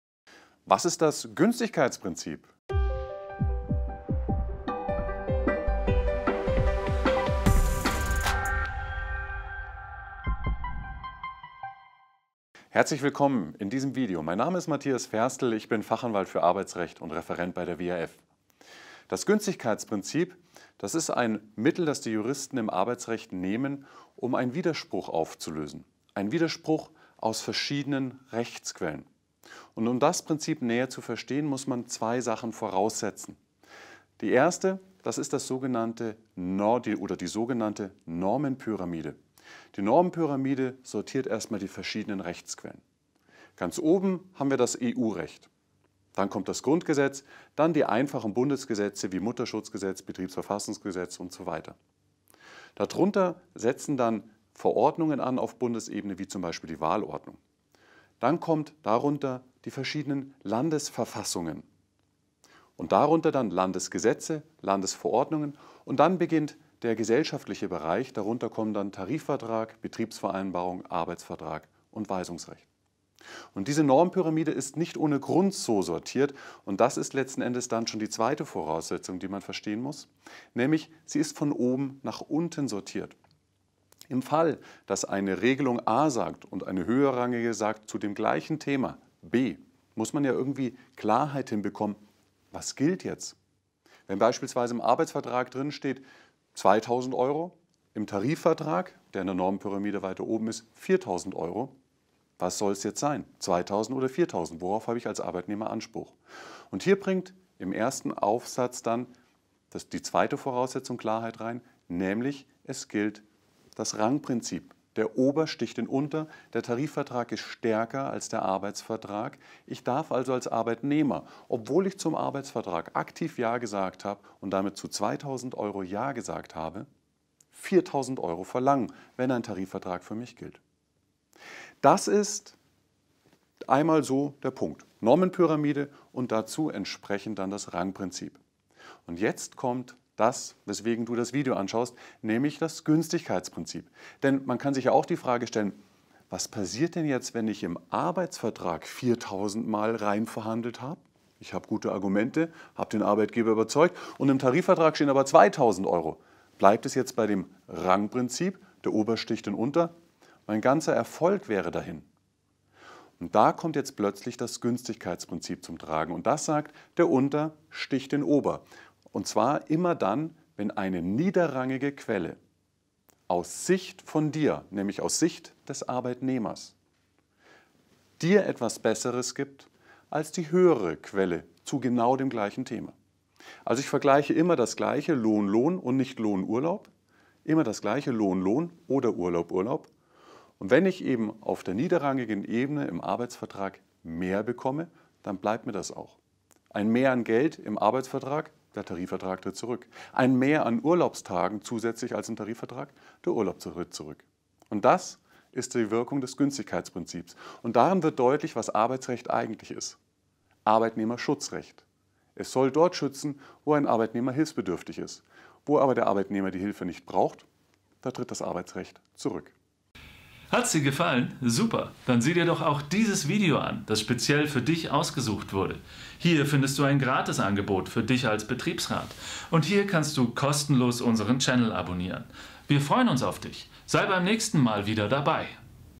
Günstigkeitsprinzip einfach erklärt: Was ist das Günstigkeitsprinzip? | Betriebsrat Video
Günstigkeitsprinzip einfach erklärt: Höherwertige Rechtsquellen haben in der Regel Vorrang vor nachgeordneten Bestimmungen. Beispielsweise dürfen Tarifverträge nicht gegen gesetzliche Bestimmungen verstoßen und Betriebsvereinbarungen dürfen keine Regelungen aus Tarifverträgen verletzen. Mehr zum Günstigkeitsprinzip gibt's im Video!

🕘 Inhalt:
0:00 - Intro
0:38 - Normenpyramide
2:22 - Günstigkeitsprinzip

SOCIAL MEDIA

WISSEN & AUSTAUSCH